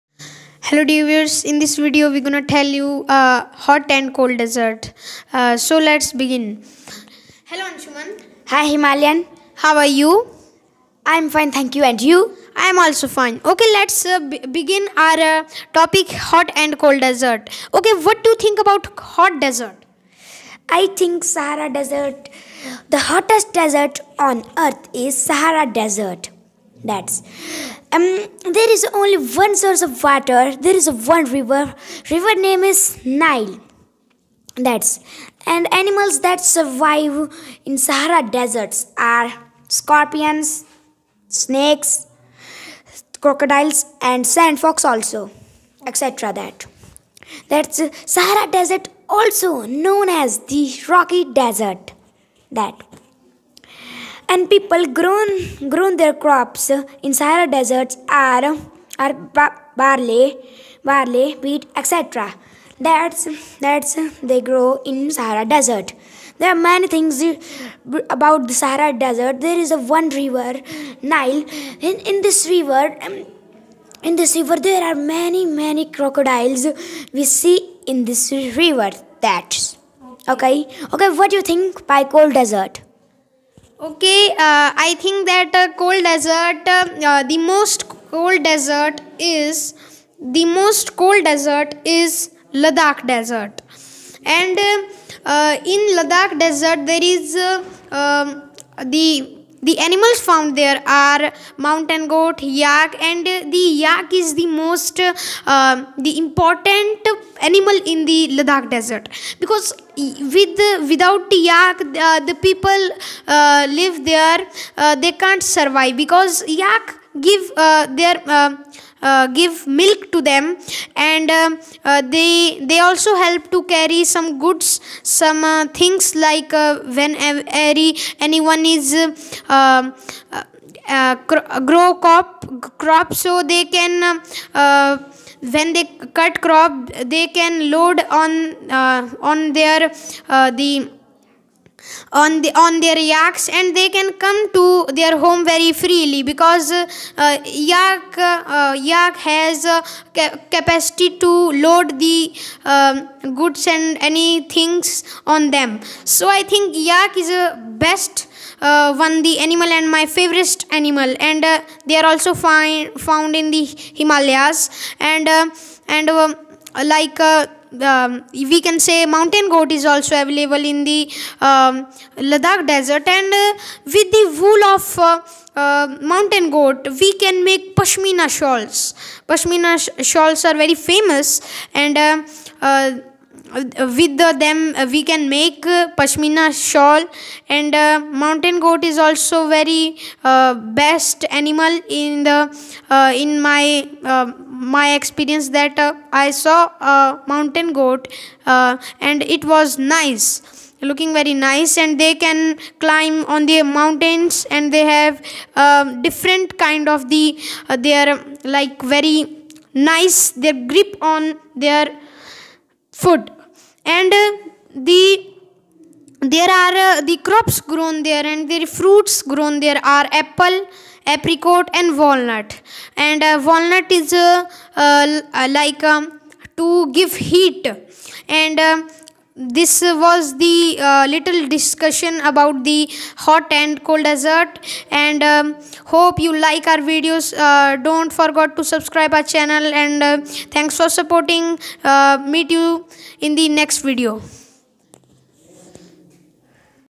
Hot and Cold Desert ||discussion by Students
Know about Hot and Cold desert
*where are the hottest and coldest desert
*the crops grown there
*the animals found there
* main rivers there
* what is importance of animals there
* all in english

  HotandColdDesert||discussionbyStudents ||